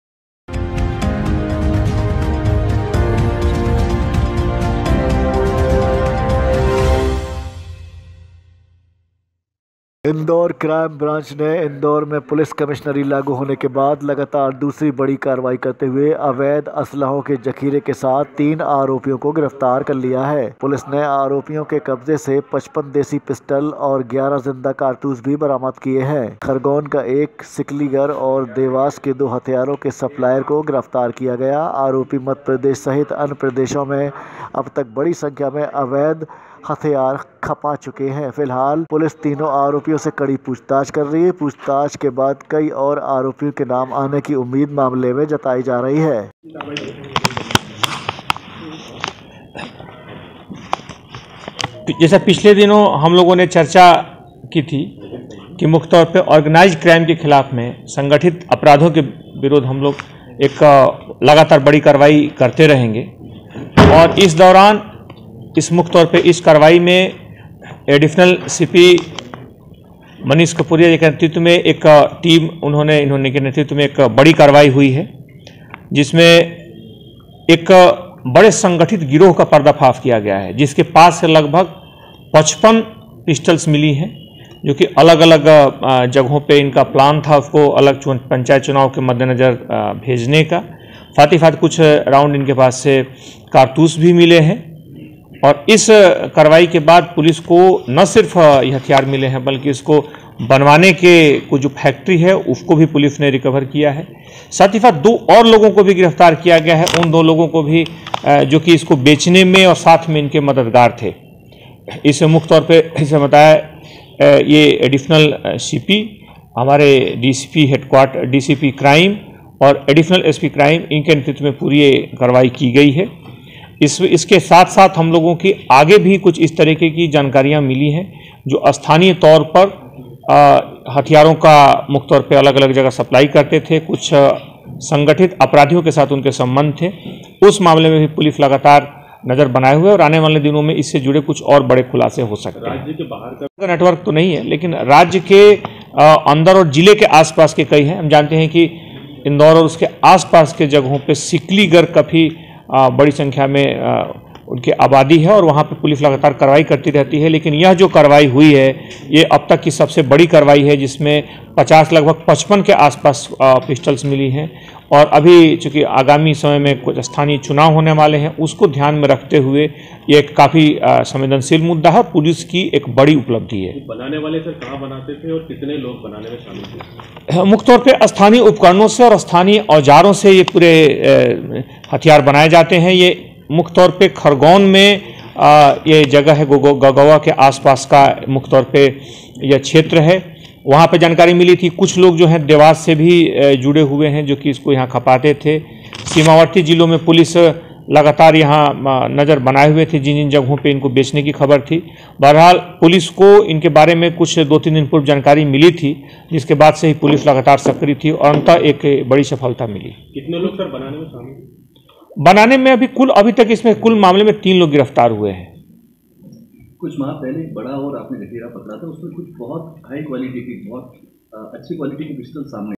इंदौर : अवेध हथियारों का जखीरा…..
इंदौर क्राइम ब्रांच ने इंदौर में पुलिस कमिश्नरी लागू होने के बाद लगातार दूसरी बड़ी कार्रवाई करते हुए अवैध हथियारों के जखीरे के साथ तीन आरोपियों को गिरफ्तार किया है पुलिस ने आरोपियों के कब्जे से 55 देसी पिस्टल और 11 जिंदा कारतूस बरामद किए हैं खरगोन का एक सिकलीगर और देवास के दो हथियारों के सप्लायर को गिरफ्तार किया है आरोपी मध्य प्रदेश सहित अन्य प्रदेशों में अब तक बड़ी संख्या में अवैध हथियार खफा चुके हैं फिलहाल पुलिस तीनों आरोपियों से पूछताछ कर रही है पूछताछ के बाद कई और आरोपियों के नाम आने की उम्मीद जताई जा रही है